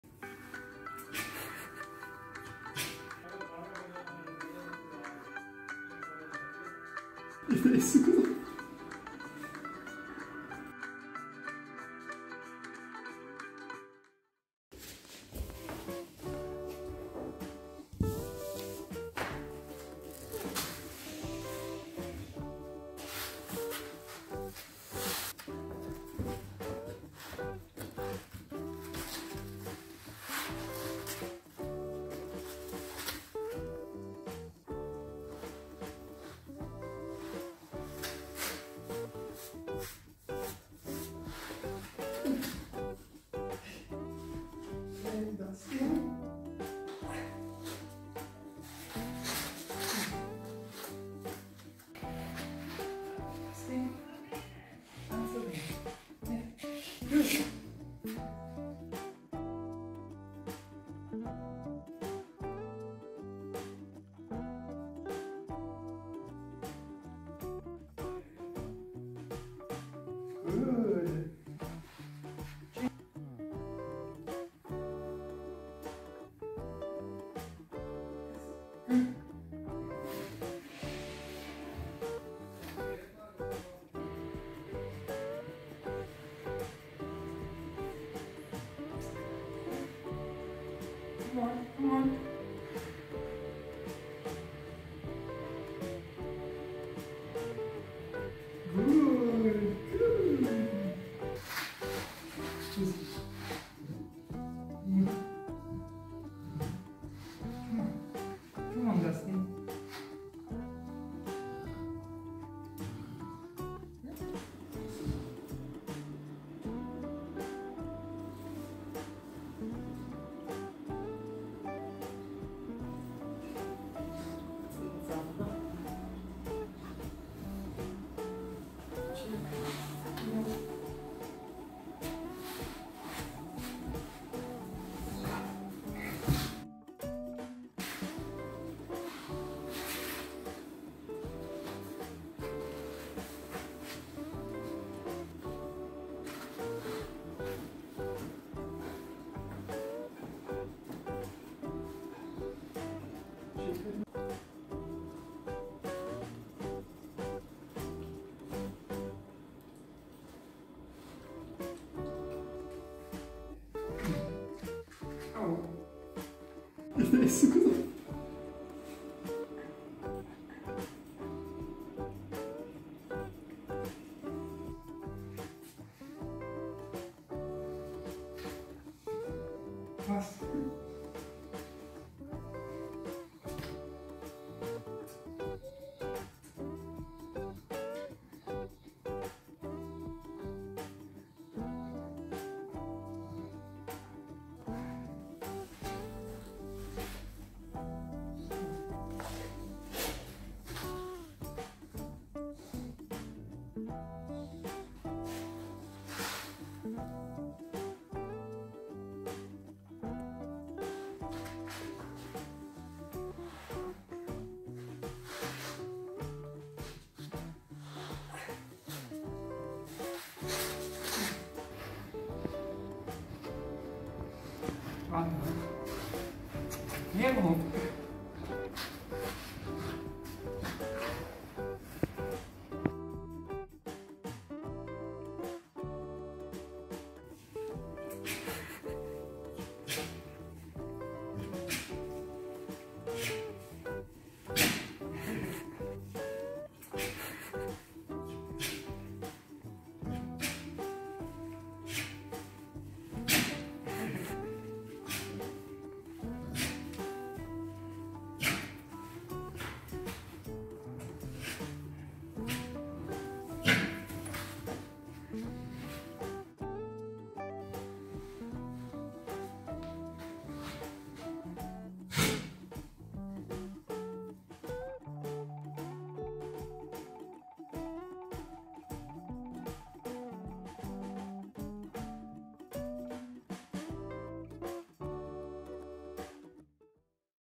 WORKING OUT WITH MY DOGS! Funny Bernese Mountain dogs reaction! Dustin&Penny
This is what happens when I try to work out at home. It's not that easy when you have 2 bernese mountain dogs! Dustin always wants to  join me but it's not that resilient, while Penny pretends... to be dead!
Now stretch out and enjoy!